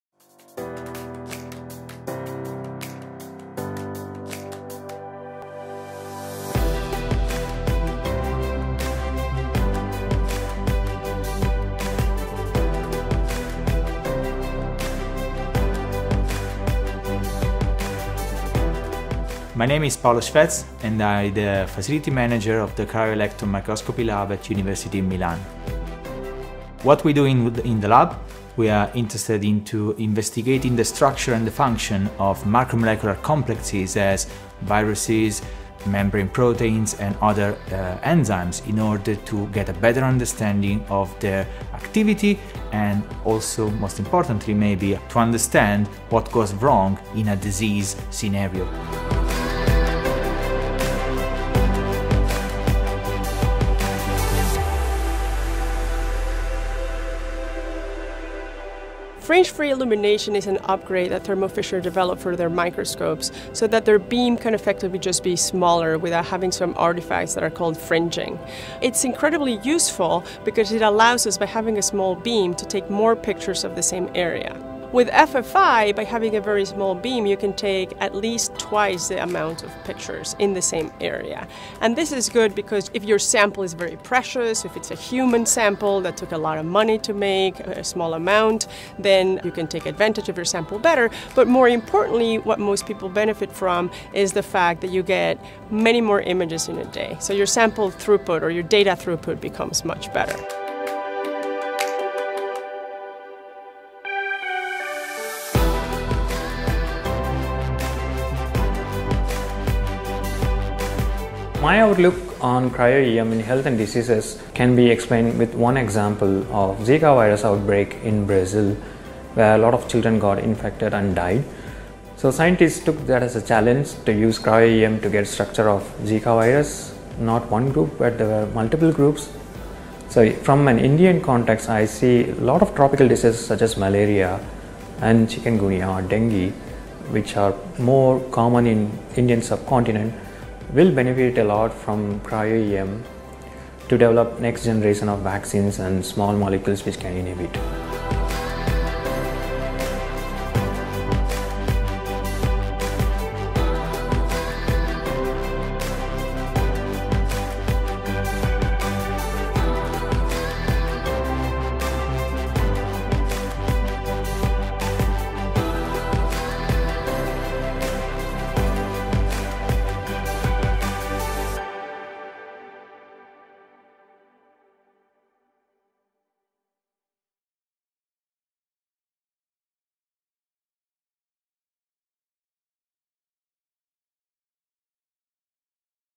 Cryo-EM: Advancing research for more users than ever before
Learn how cryo-EM is helping to study the structure and function of macromolecular complexes and viruses in understanding disease progression. This will, in turn, help to design drugs that can help patients with various diseases.